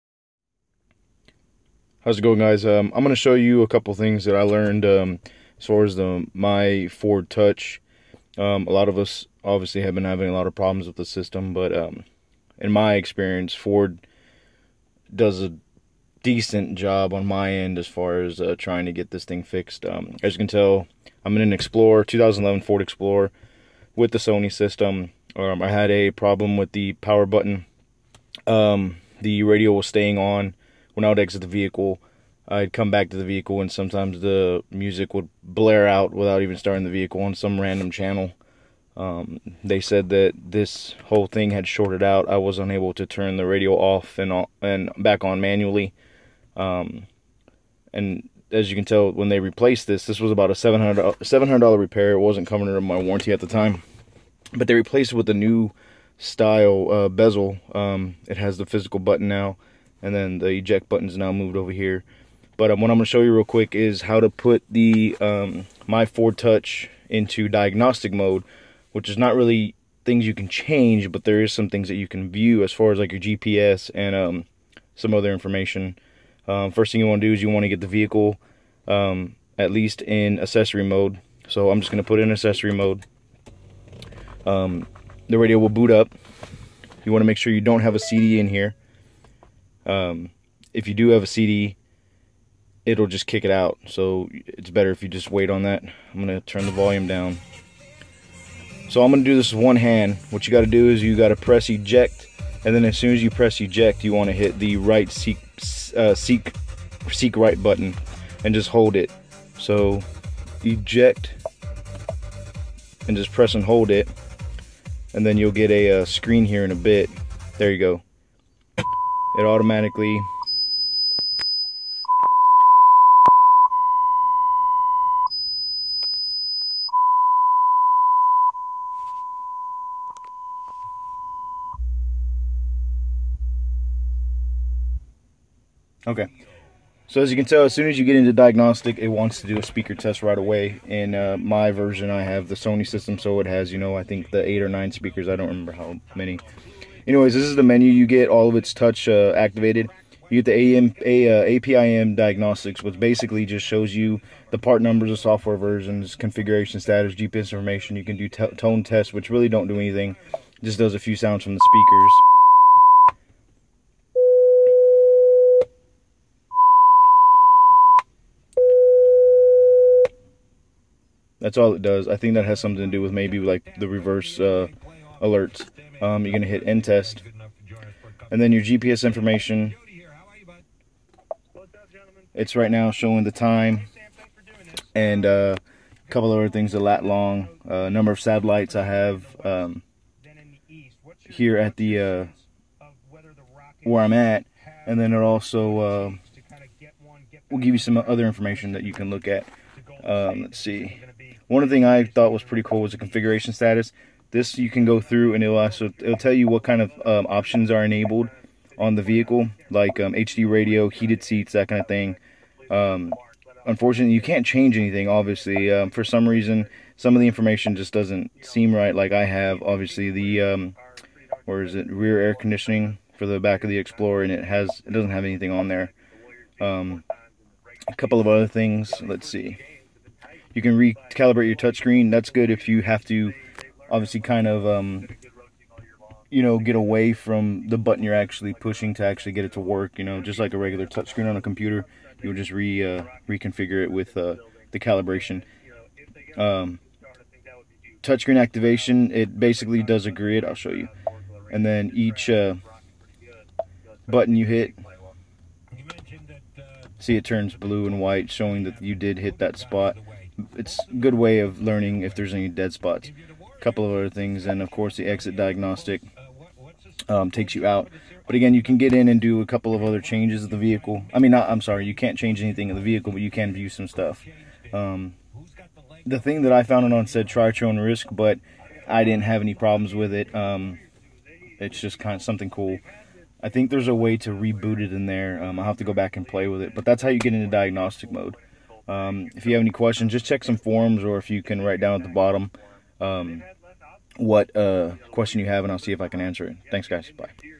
MyFordTouch Diagnostic Mode
This shows how to enter the diagnostic mode on the MFT system in a 2011 Ford Explorer Limited. This should work with other models with the same system.